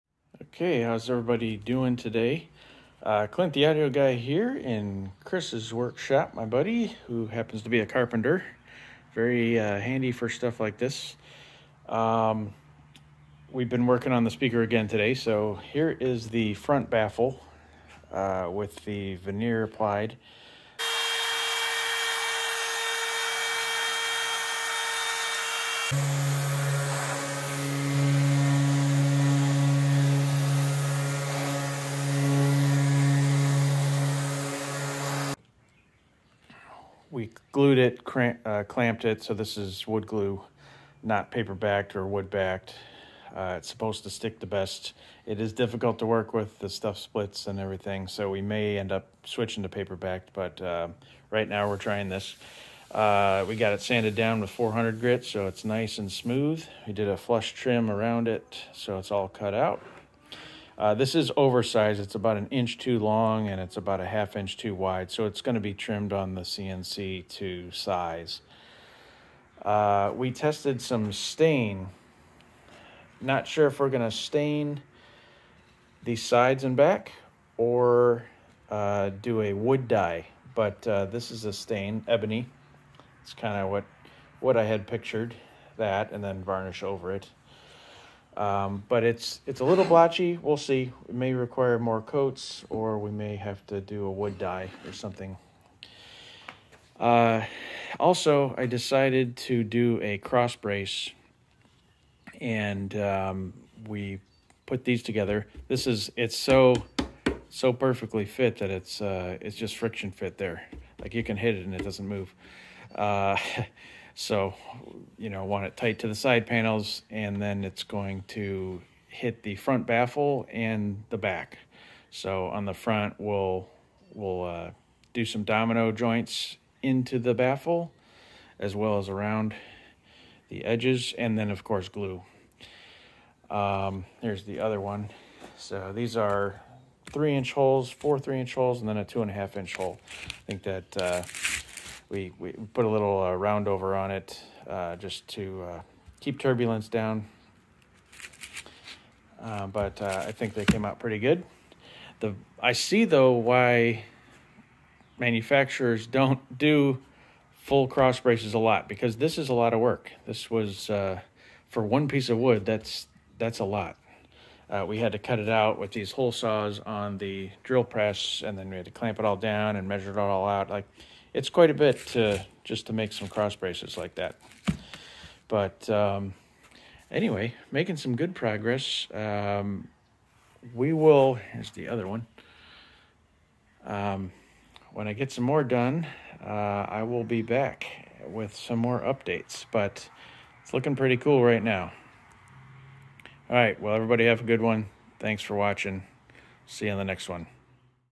Speaker Project Part 3. The Crossbracing and Veneer
Just showing some more progress on the speaker project. We figured out a cross brace, and the worked on the veneer and tested an ebony stain finish.